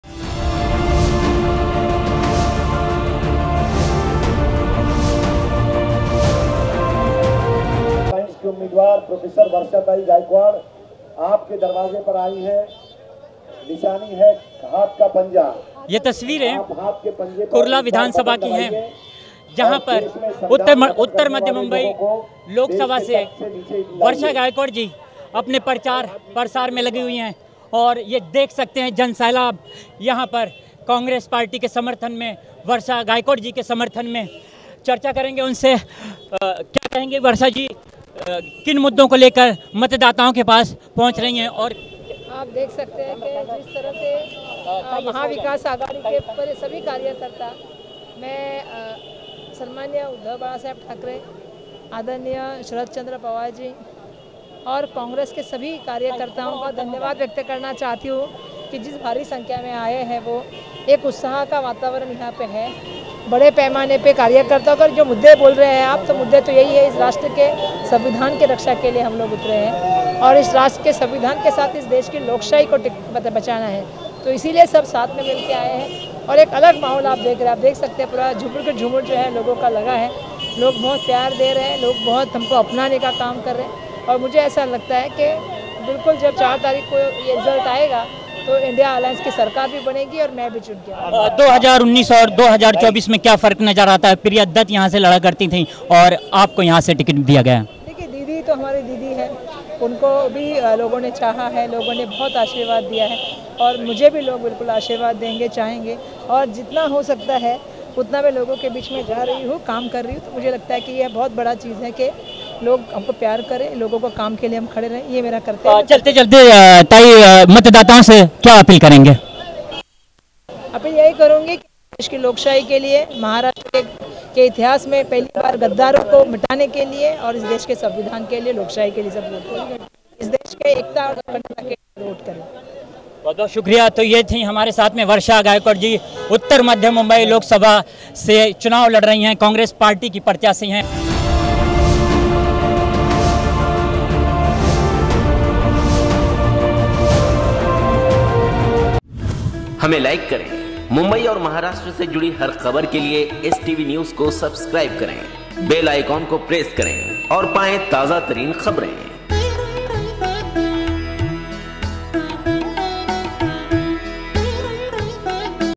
वर्षा गायकवाड के चुनावी प्रचार मे कुर्ला मे उमड़ा जन सैलाब इंडिया गठबंधन के सभी दल मैदान मे
वर्षा गायकवाड के चुनावी प्रचार मे
कुर्ला मे उमड़ा जन सैलाब इंडिया गठबंधन के सभी दल मैदान मे  Loksabha Election 2024